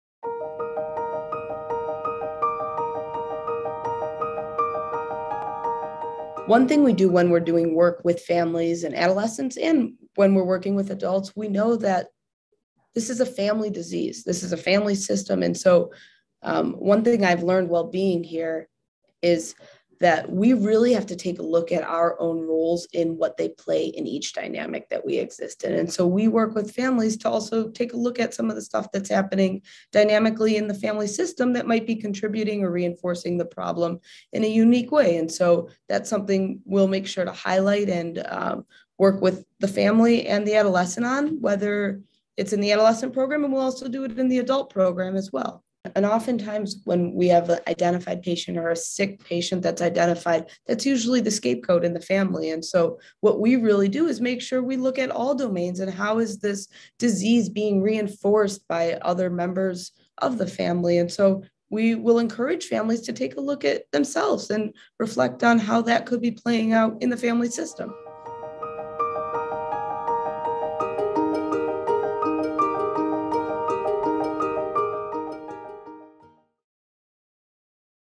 Dr. Nicole Bishop, Psychologist and Clinical Director on the role of the family unit in treatment.
Dr. Nicole Bishop, Psychologist and Clinical Director on the role of the family unit in the treatment process.